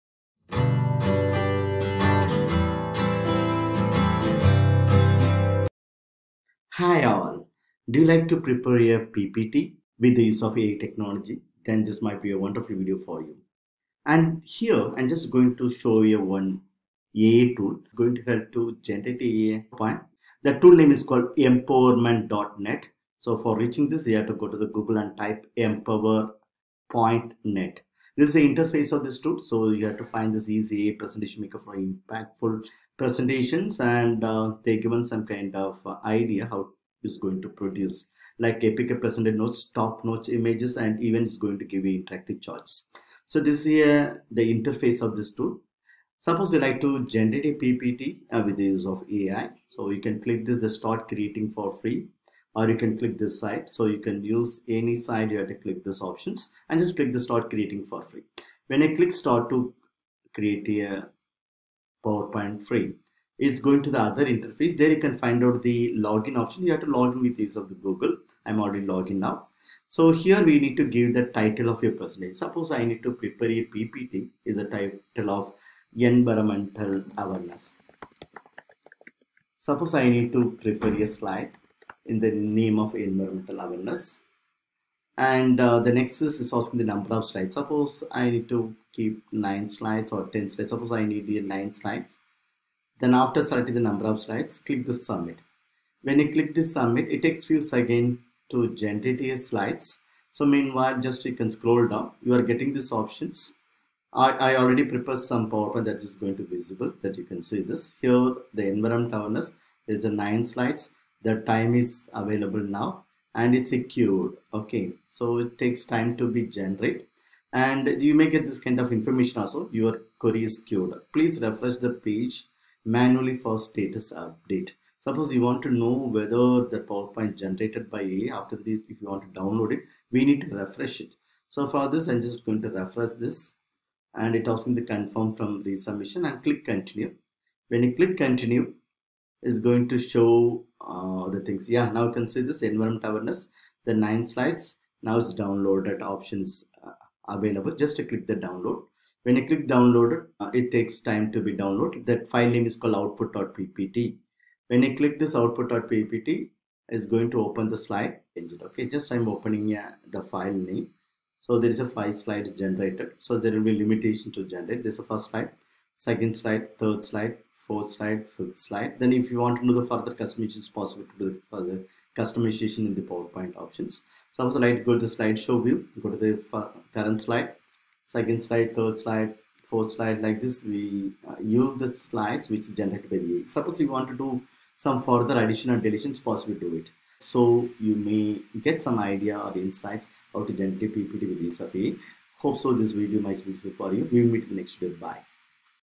Master Your Presentations with Empowerpoint: The Ultimate AI Presentation Maker!
Welcome to our tutorial on Empowerpoint, the cutting-edge AI presentation maker that will transform the way you create slideshows! In this video, we’ll walk you through the features and functionalities of Empowerpoint, showing you how to effortlessly design professional presentations. Whether you're a student, teacher, or professional, Empowerpoint is your go-to tool for crafting engaging and visually stunning presentations in no time.

What You'll Learn:

How to get started with Empowerpoint
Key features and tools
Tips for creating dynamic and interactive slides
Best practices for using AI to enhance your presentations